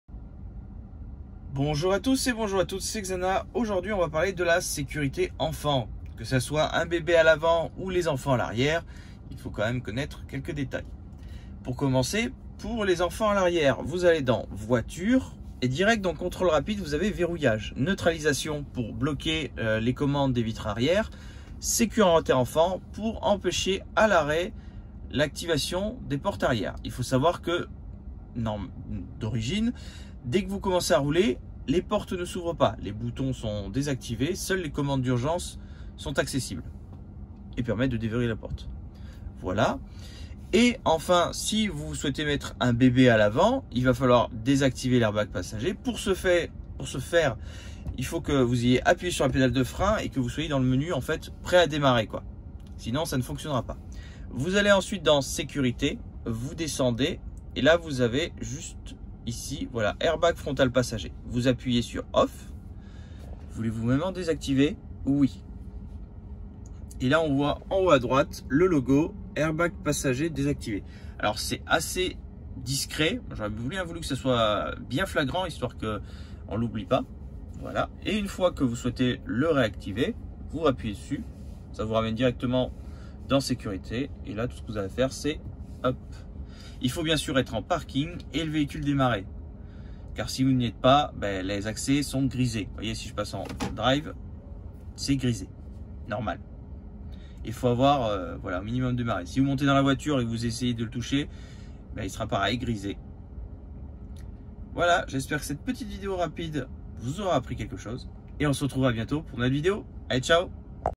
Tesla Model 3/Y Sécurité enfant/bébé
Dans cette vidéo comment désactivé l'airbag passager et désactivé les fenêtres et portes arrière

Site Web pour tout ce qui concerne la Tesla